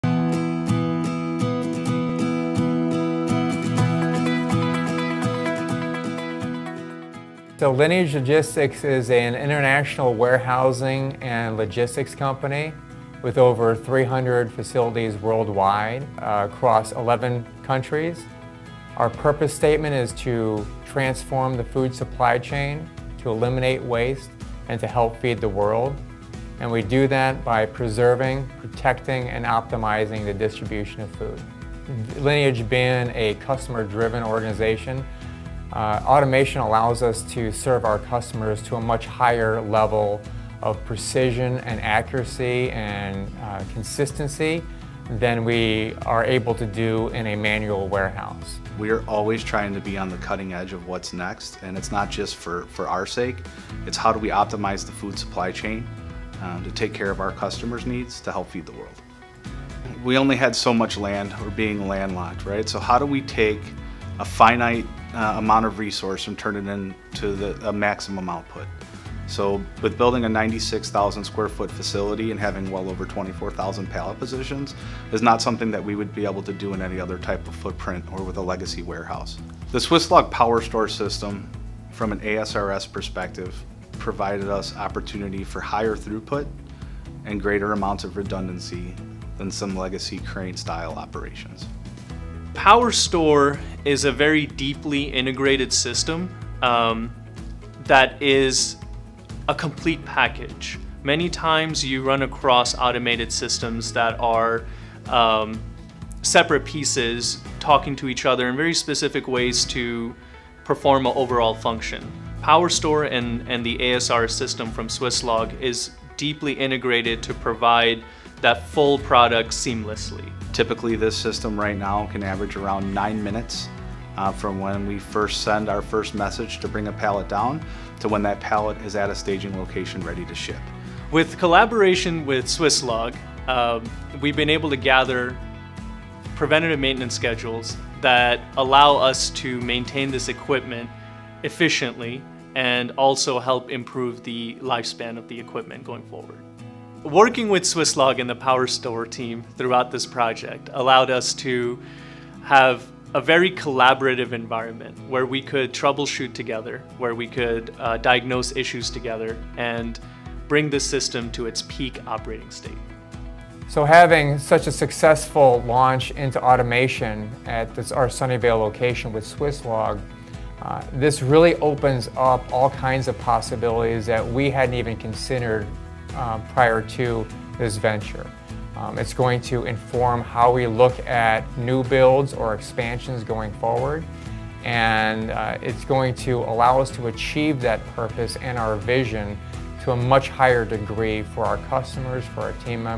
Lineage Logistics, USA: Surpassing expectations in next generation cold storage facility
Lineage Logistics, a global leader in cold storage, is committed to being on the cutting edge of supply chain technology to meet the constantly evolving needs of its customers. As a customer-driven organization they needed to optimize their food supply chain and establish their first fully automated cold storage facility in North America.

At the heart of the Lineage facility is Swisslog's PowerStore automated storage and retrieval system. The compact, high density pallet shuttle system is capable of meeting high throughput requirements while minimizing worker exposure to the harsh conditions in the cold.

They chose Swisslog’s PowerStore automated storage and retrieval system to be the centerpiece of the facility, providing maximum flexibility, efficiency and responsiveness.

Hear Lineage Logistics executives explain how Swisslog is helping the company transform the food supply chain, eliminate waste and help feed the world

Follow Swisslog for the latest insights into robot-based and data-driven intralogistics solutions.